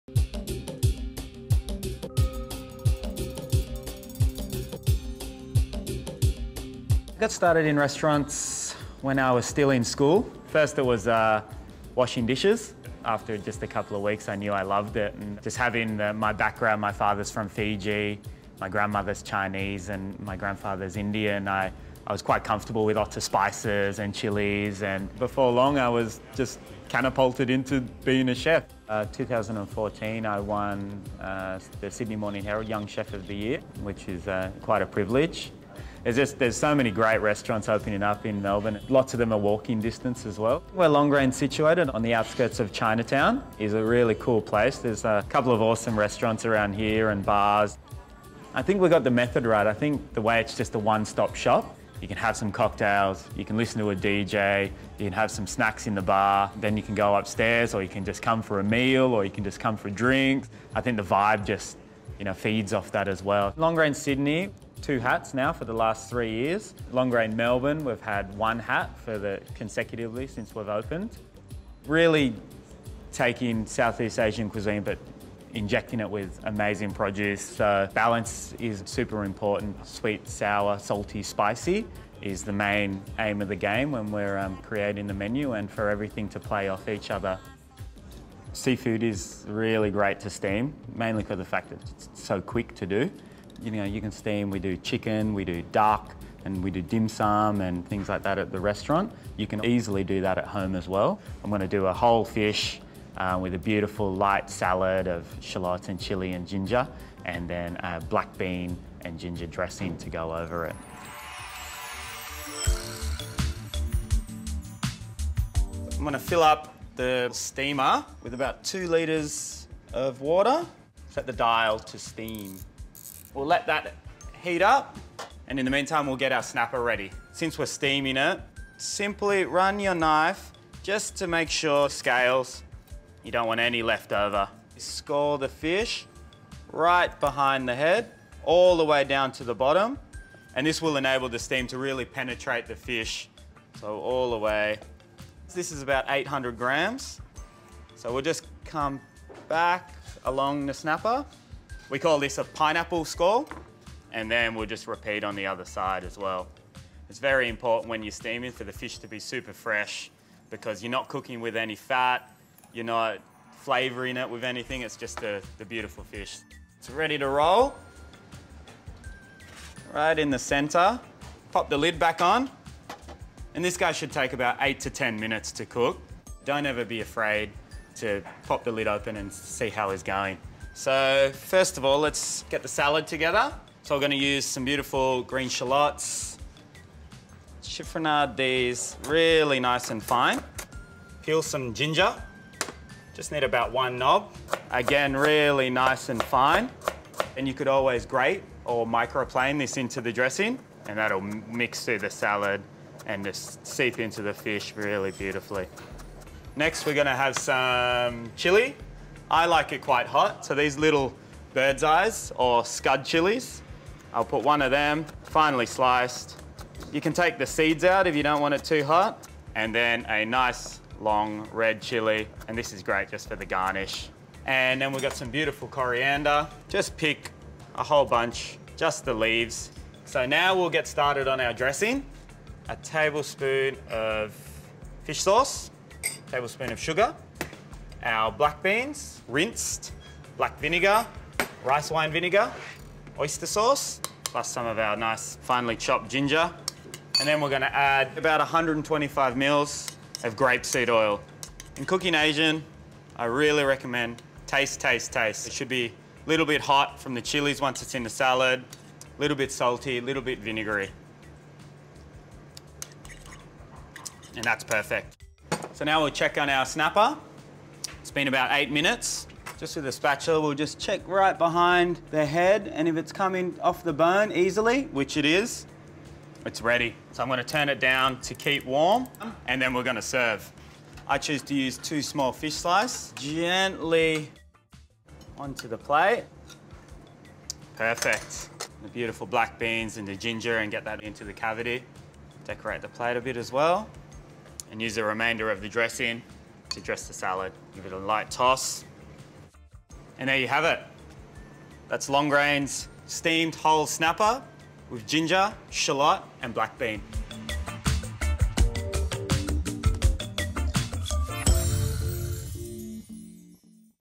The Best Steamed Snapper Recipe Ever - By Longrain and Breville Australia
Longrain's exec Chef, Louis Tikaram shows you how to make Longrain's signature steamed snapper with ginger,shallot and black bean in the Breville Hot Wok & Steam.

Longrain's exec Chef, Louis Tikaram, who recently scooped the prestigious Josephine Pignolet Best Young Chef of the Year award, credits his culinary inspiration from a childhood spent growing up surrounded by fresh produce on 100 Acres of farmland in Mullumbimby, NSW and his Fijian-Chinese heritage.

Louis completed his apprentice to Longrain founding-chef, Martin Boetz at Longrain Sydney, he then joined the opening team at The Bentley restaurant, and went on to hone his skills at Tetsuya's. Then after working at some of the top kitchens in America, Spain and South East Asia, he returned to Sydney to take the reins as Longrain's Head Chef. Louis brings his own interpretation of modern South East Asian food to Longrain, continuing the legacy of his mentor Martin Boetz.